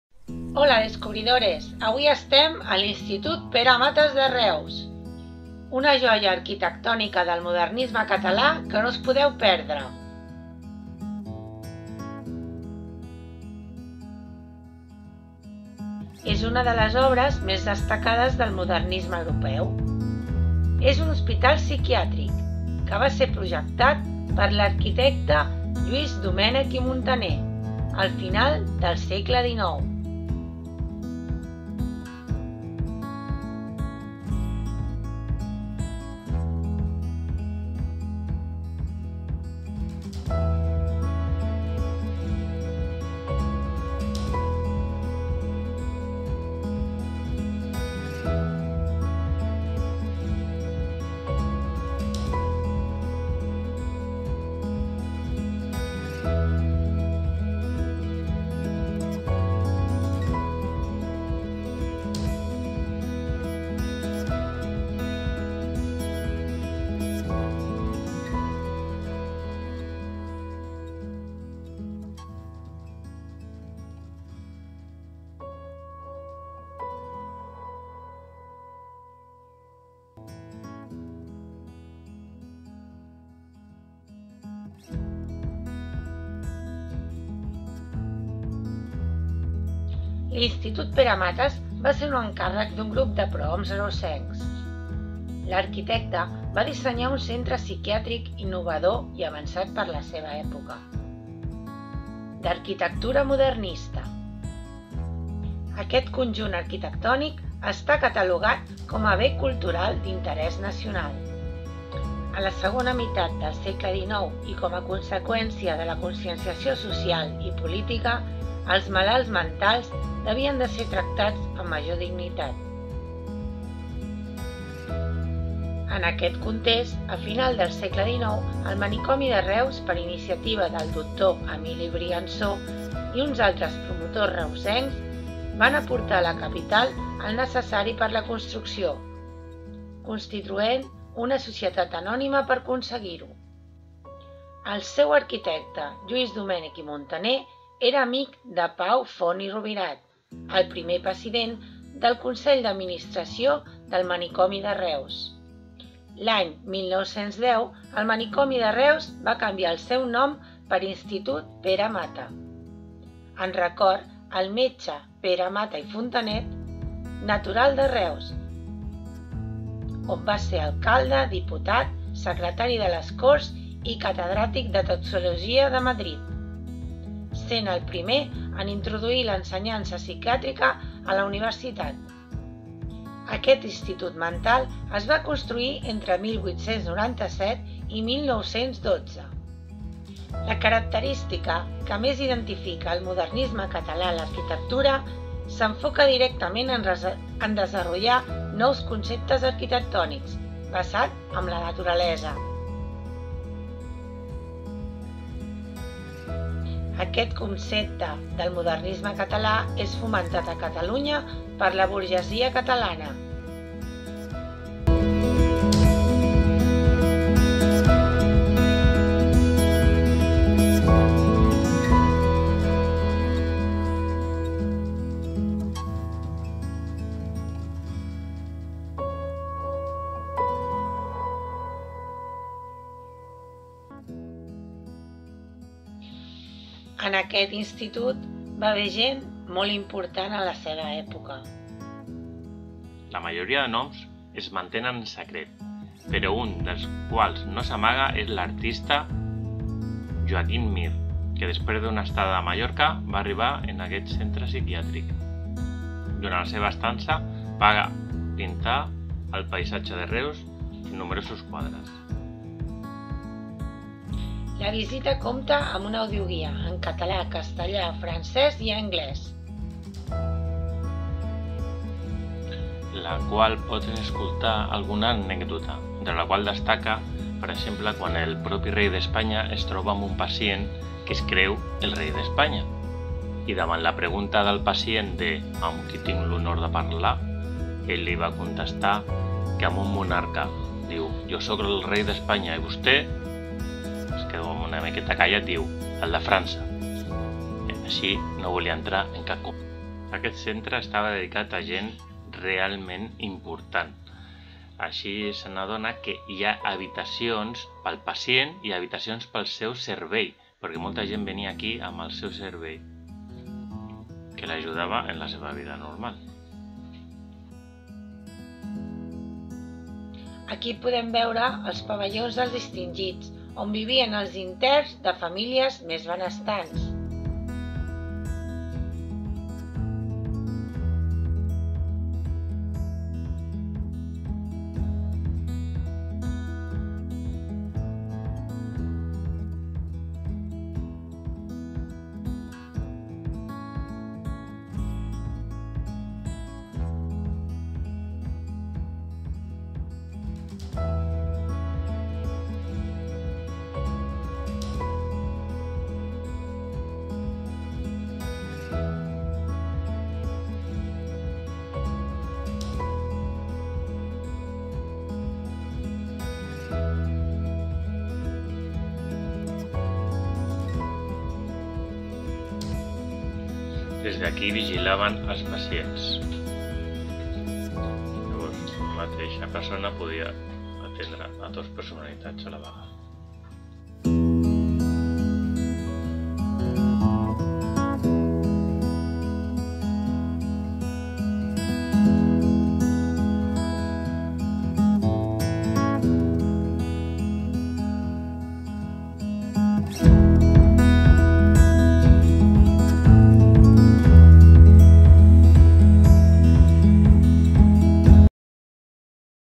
L'Institut Pere Mata de Reus
L'Institut Pere Mata de Reus és l'hospital psiquiàtric.Va ser projectat per Lluís Domènech i Montaner i construït entre 1897 i 1912, en l'estil modernista propi d'aquest arquitecte. El nom és un homenatge al metge reusenc Pere Mata i Fontanet. El conjunt arquitectònic modernista està catalogat com a BCIL i, específicament, el pavelló dels Distingits és un Bé Cultural d'Interès Nacional des del 2013. ___
Visita HOLA DESCUBRIDORES en un mapa